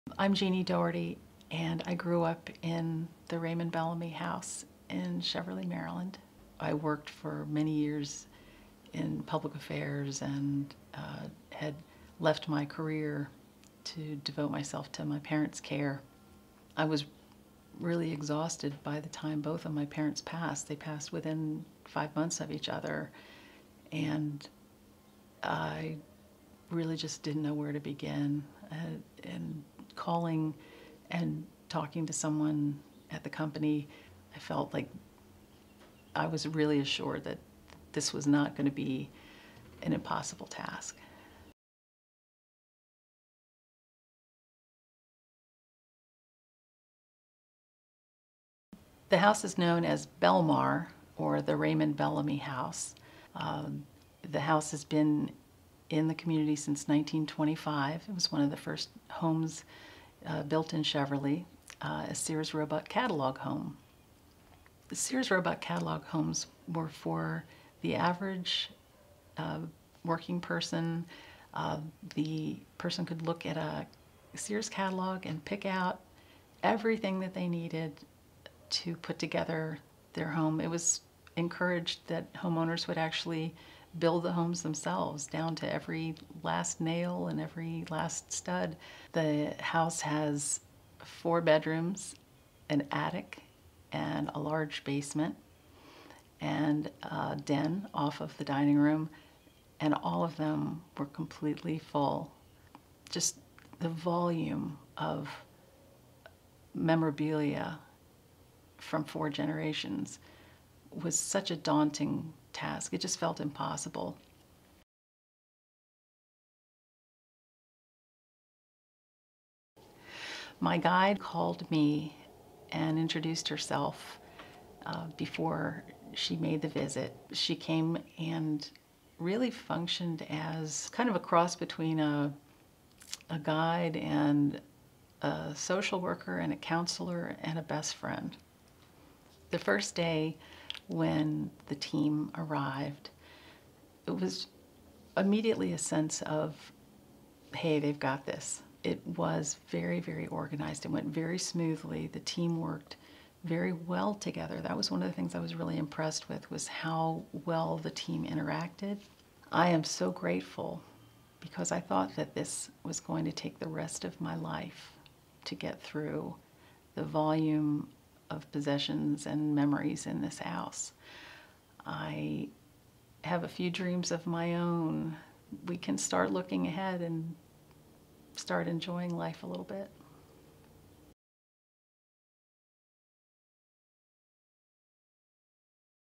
Jeanne Turned to WayForth For Help With Her Parents' Estate Liquidation
Jeanne Doherty left her career to become a full time caregiver for her parents. After their death, she was left to handle the historical Sears catalog home and the items within it.  Four generations of memorabilia filled the home and needed special care. Overwhelmed by the tasks ahead, she contacted WayForth. With one call Jeanne got her life back.